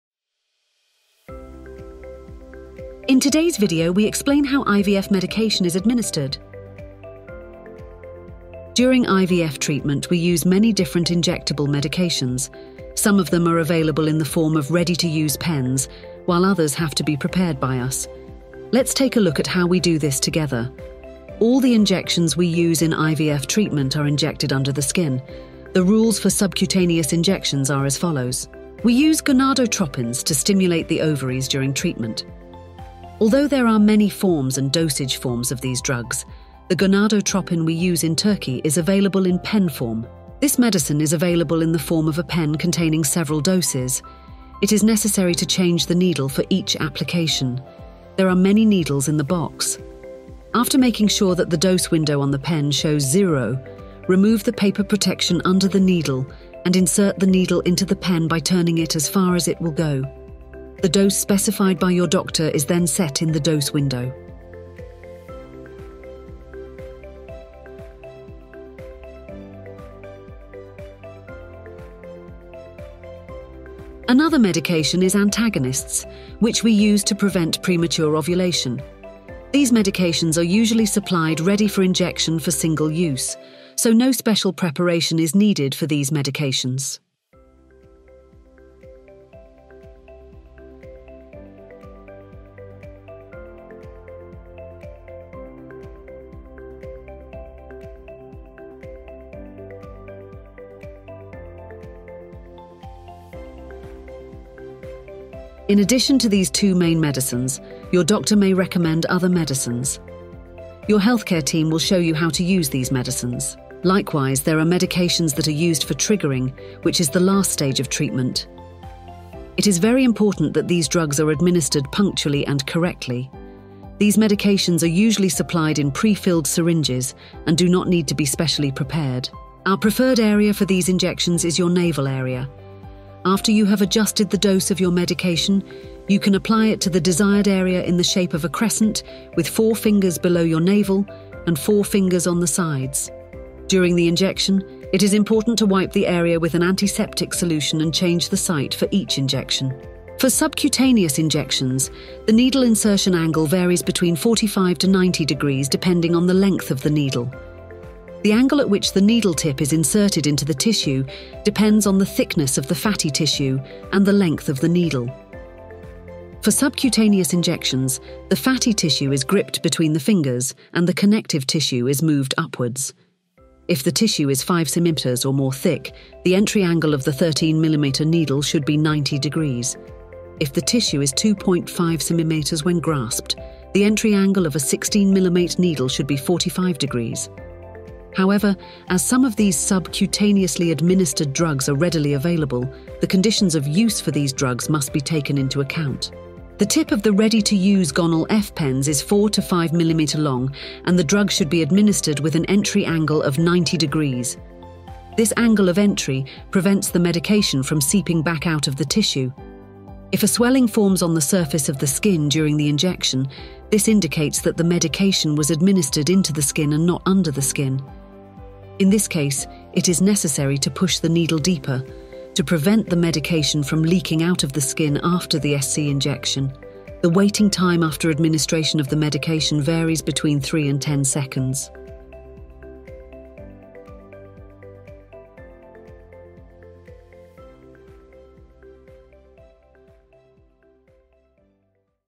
Easy Guide: How to Administer IVF Medications
Welcome to our fertility and IVF treatment channel! In today's video, we're sharing a straightforward and easy-to-understand guide on administering medications for In Vitro Fertilization (IVF).

What you'll find in this video:

Understanding the different types of medications used in IVF.
Step-by-step instructions for using injectable pens.
Tips for subcutaneous injection around the navel area.
The importance of the injection angle and how to determine it.
Answers to common questions and tips for effective administration.

Clear explanations suitable for everyone, whether you're new to IVF or have some experience.
Practical advice to ease your IVF journey.
Visual aids for better understanding of injection techniques.
Don't forget to:

👍 Like the video if you find it helpful.
💬 Leave your questions or comments below. We're here to help!